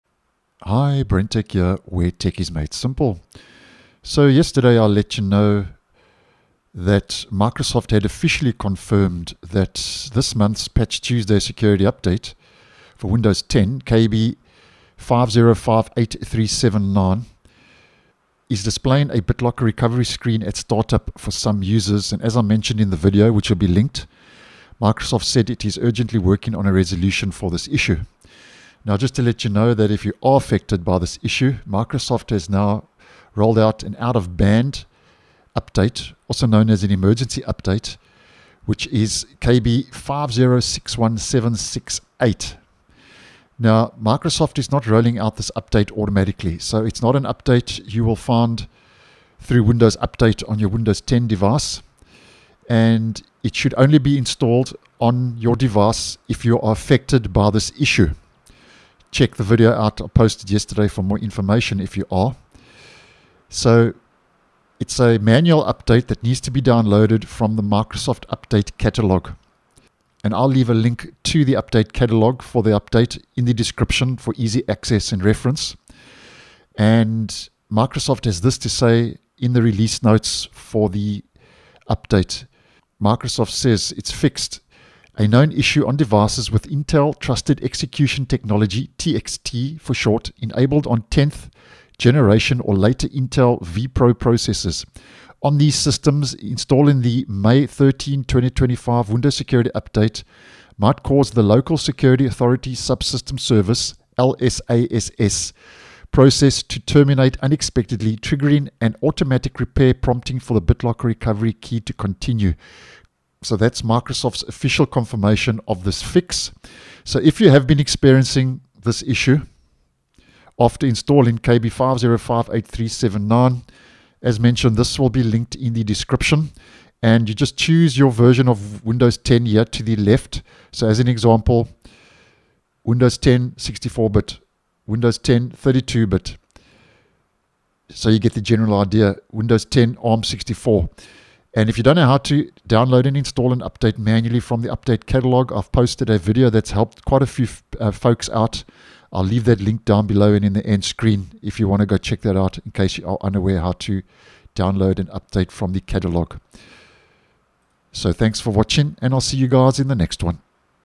Windows 10 Emergency Update KB5061768 Released Fixing BitLocker Recovery Problems!
Microsoft is not rolling out this update automatically and it should only be installed on systems affected by this issue. The update needs to be manually downloaded and installed from the Microsoft Update Catalog